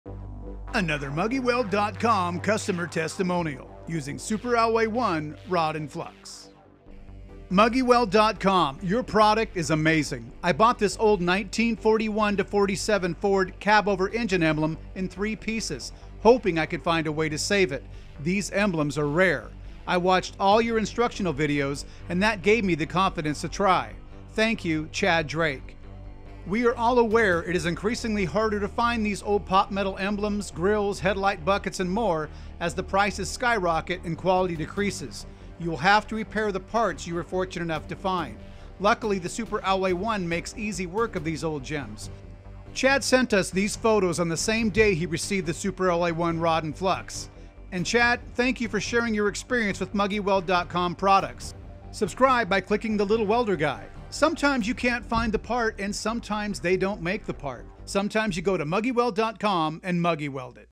1941-47 Ford Pot Metal Cabover Engine Emblem Repaired with Super Alloy 1
Super Alloy 1 contains cadmium. another Super Alloy 1 customer testimonial.

MuggyWeld.com, your product is amazing! I bought this old 1941-47 Ford Cabover Engine (COE) emblem in 3 pieces hoping I could find a way to save it. These emblems are rare. I watched all your instructional videos and that gave me the confidence to try. Thank you. Chad Drake.

We all know it will be harder to find these old pot metal emblems, grilles, headlight buckets, and more as the prices go up and the quality goes down. They don’t make aftermarket pot metal parts for most makes and models of classic cars. You will have to repair the parts you are able to find. Luckily the Super Alloy 1 makes easy work of these old gems. Watch the video in the upper right corner to see how simple it is to restore “so-called” impossible to repair treasures. These photos were sent to us the same day the product was delivered to Chad. They can be chrome plated if he chooses to do so. Thank you, Chad for your photo testimonial.

Clean the pot metal before soldering. Pot Metal can start to oxidize within hours of cleaning. It is possible to solder next to chrome plating without blistering the plating. The Super Alloy 1 will take powder coating. The solder will work on missing sections and can be used to build up. Back off with the flame to layer the solder upon itself. Flux the base pass and the last pass when building up—no need to flux between layers of the build-up. If the flux turns black, stop heating and clean the burnt flux off with warm water and a wire brush.
Clean the flux off of the rod before placing it back into the PVC tube.